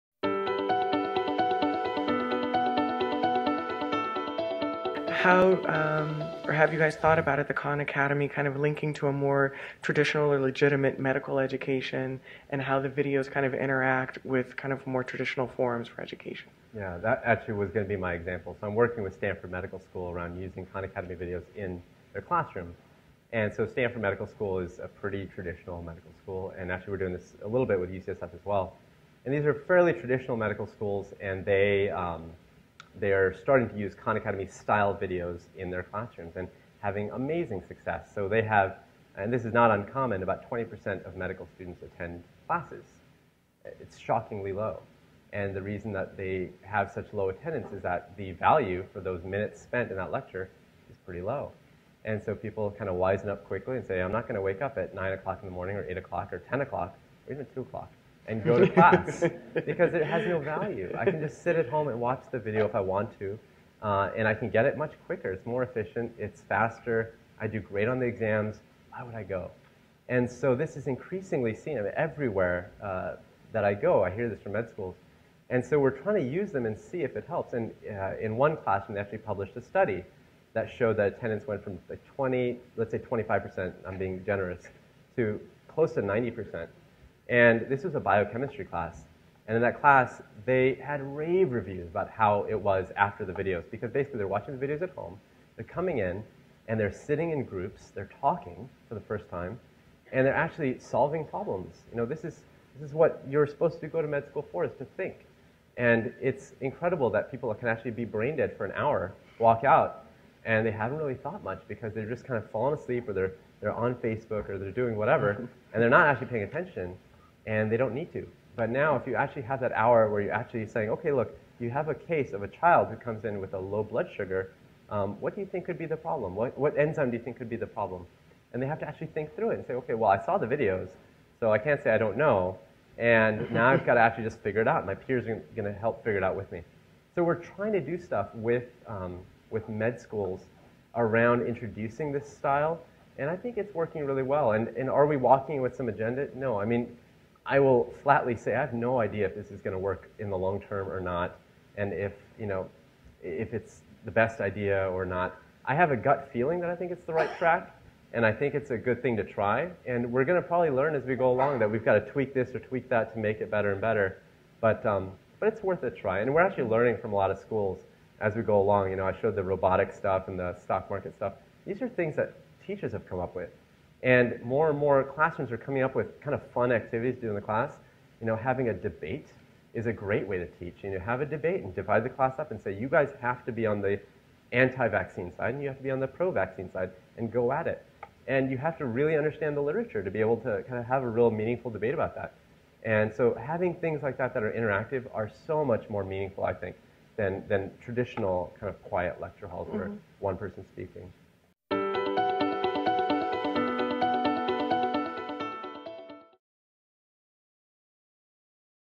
KHAN ACADEMY Can Khan Academy Integrate with Traditional Medical Education
Rishi Desai, MD, a Khan Academy Medical Fellow, visited CenterScene on January 24, 2013 to discuss how the Khan Academy is changing how we learn.  The Khan Academy, a free online classroom offering courses in math and humanities , has expanded its curriculum to offer illustrated, easy-to-understand medical lessons to transform the way we approach our wellness.

Dr. Desai is helping to democratize medical and healthcare education and discusses how widespread access to this curriculum will empower us at our doctor's office and at home, as well as expand access to healthcare in our communities.

Rishi Desai is trained as a pediatric infectious disease physician and has worked at prestigious academic centers across the country. Desai has always had a love for learning and has taught students at every stage of his career: he has won numerous teaching awards for medical education, and his passion for teaching brought him to the Khan Academy. Founded by Sal Khan, the Khan Academy provides self-pacing software and unlimited access to over 3,000 instructional videos on its YouTube channel. The website and library of educational videos—one of the most-used libraries on the web—offers lessons on everything from basic arithmetic to college level science and economics, humanities, test prep and now, medical and public health information. Since joining the team, Desai has worked with multiple medical schools to implement online video-based content directly into their curriculum.

Desai had an accelerated early education, completing high school and receiving his BS in Microbiology from UCLA by the age of 18. He also spent two years working at the Centers for Disease Control and Prevention as an Epidemic Intelligence Officer, where he worked on disease outbreak investigations.